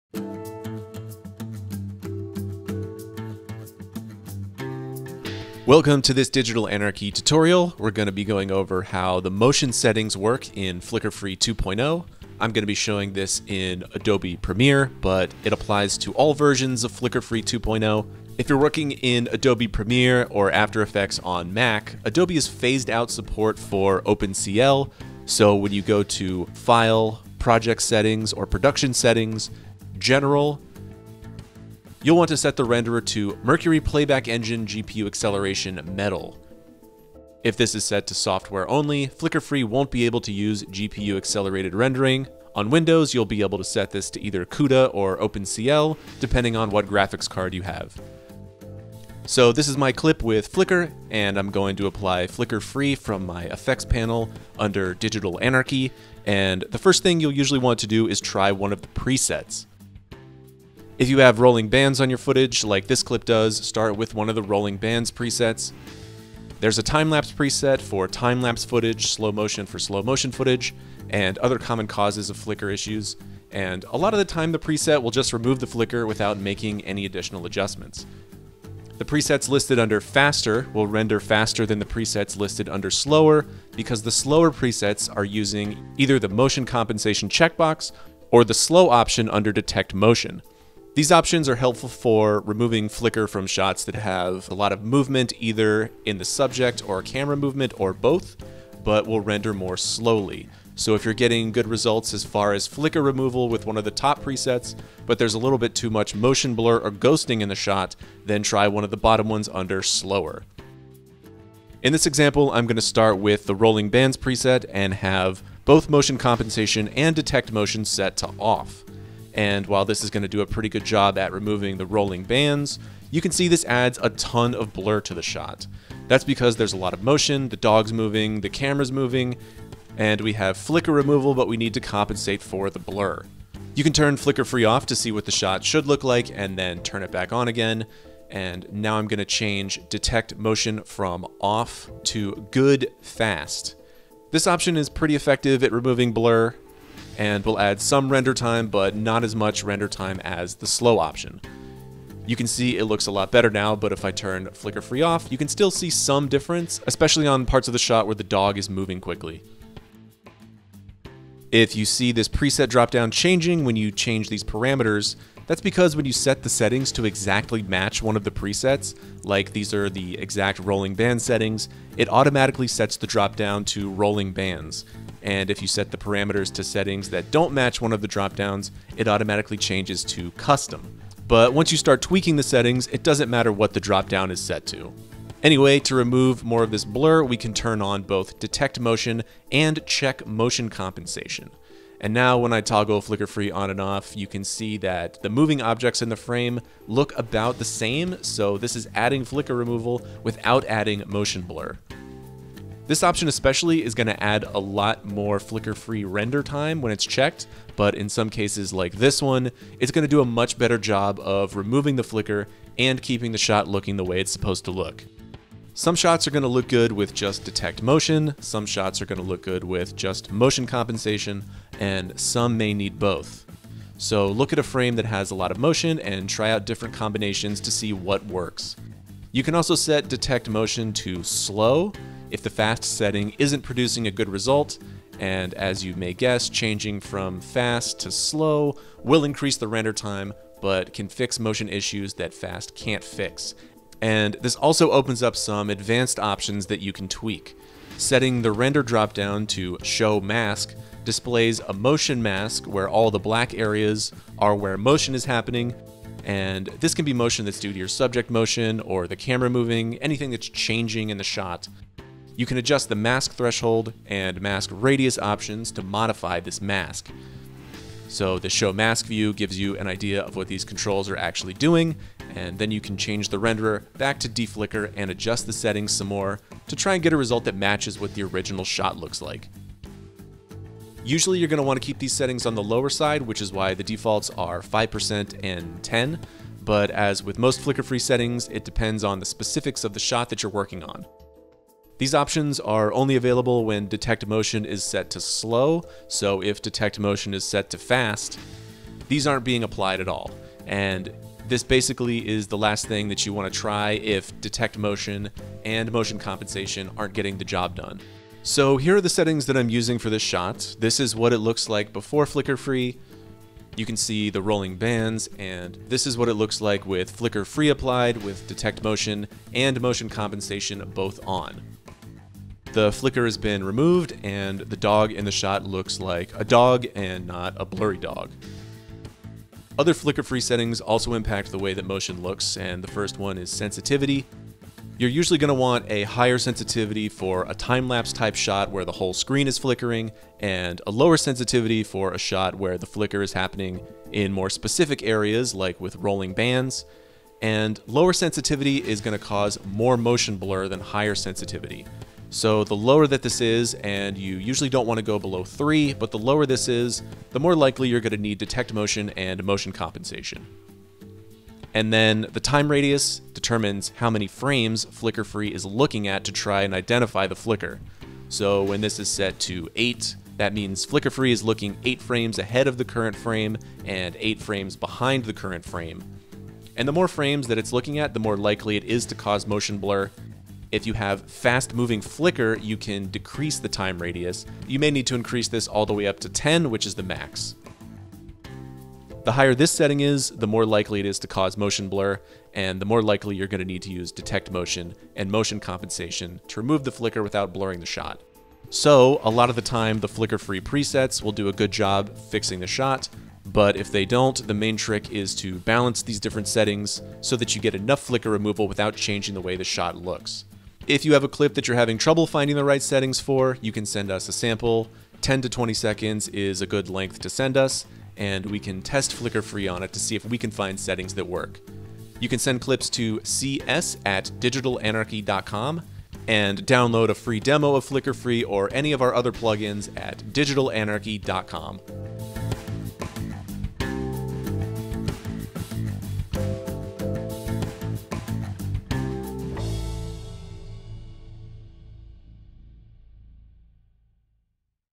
Flicker Free 2.0: Removing Flicker from Shots with Motion
Motion compensation allows Flicker Free 2.0 users to remove flicker from footage that was previously unfixable. The newest version of the plugin uses optical flow algorithms to detect and delineate independently moving objects, even in the presence of camera motion. Learn more about motion compensation and how you can better utilize this option to remove flicker from your video in this tutorial.

Learn more about  motion compensation and how you can better utilize this option to remove flicker from your video in this tutorial.

To try Flicker Free for yourself, download a demo!